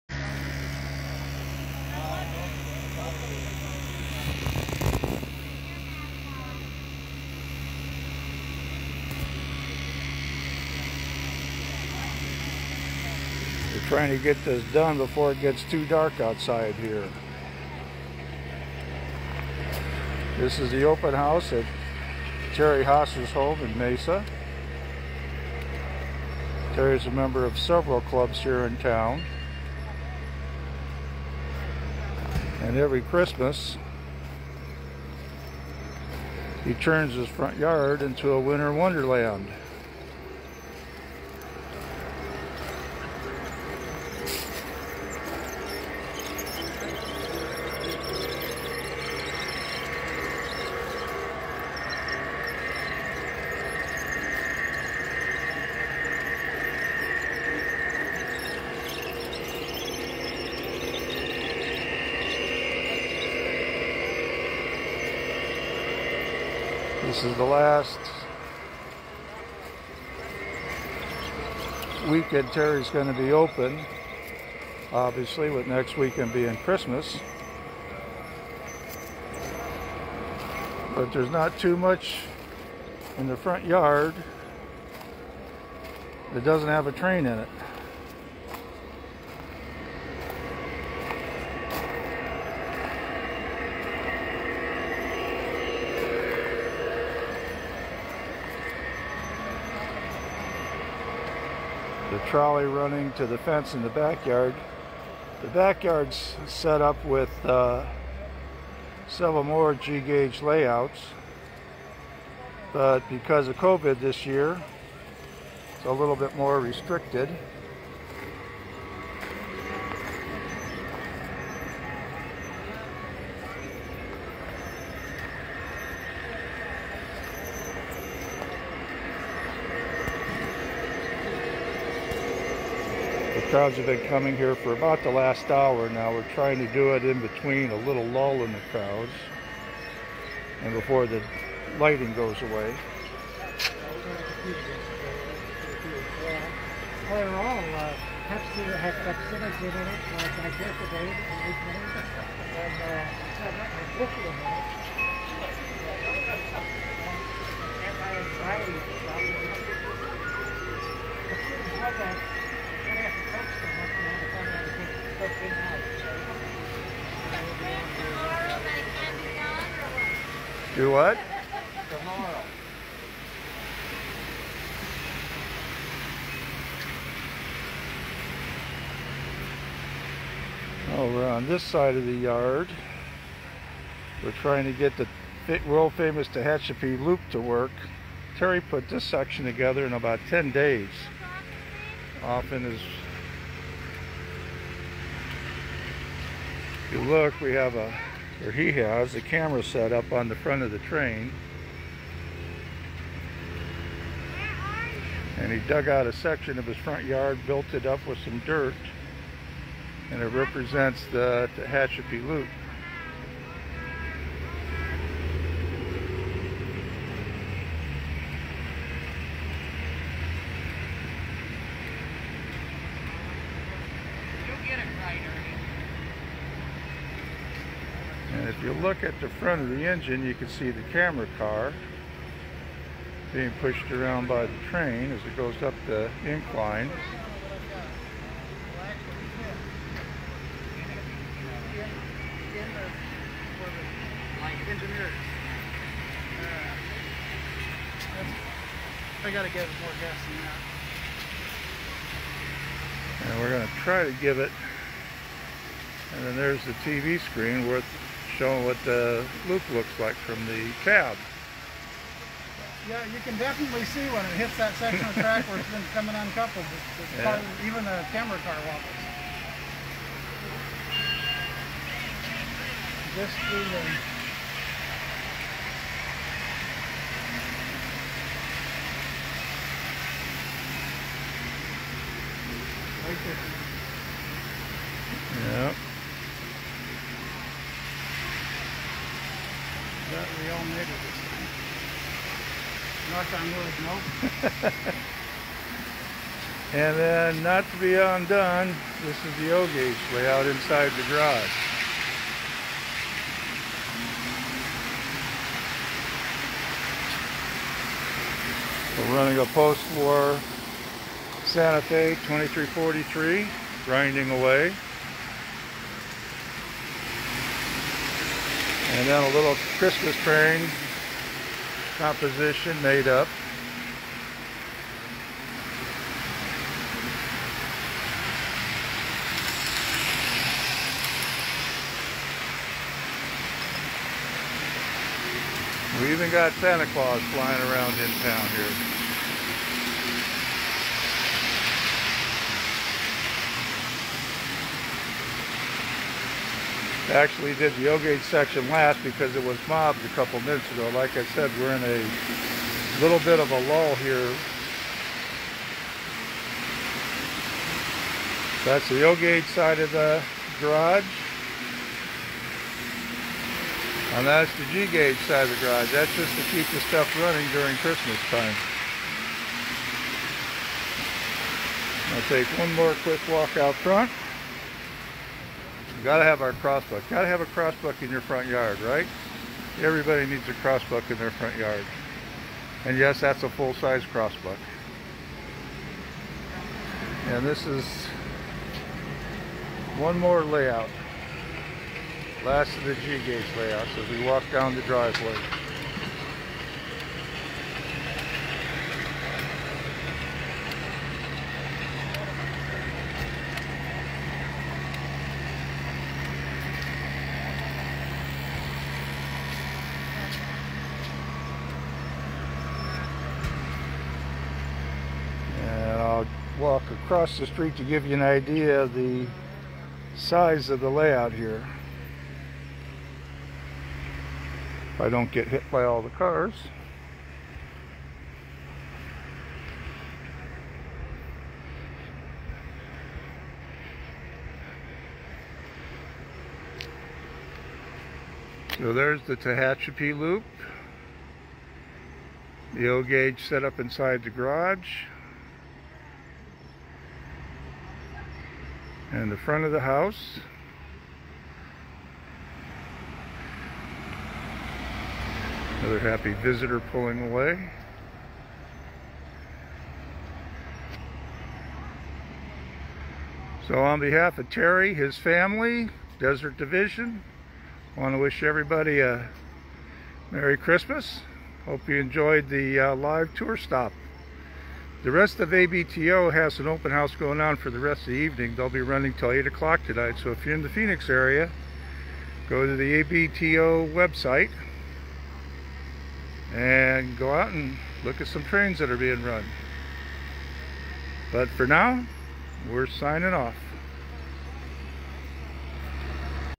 Terry Haas Open House December 2020
A quick walk around the front yard of Desert Division member Terry Haas during his open house held December 5th, 6th, 12th and 13th in Mesa, AZ. Terry is a member of several train clubs in the area and the ABTO members hold several open houses twice a year to support club activities. You can visit about twelve different layouts over the two-day weekend.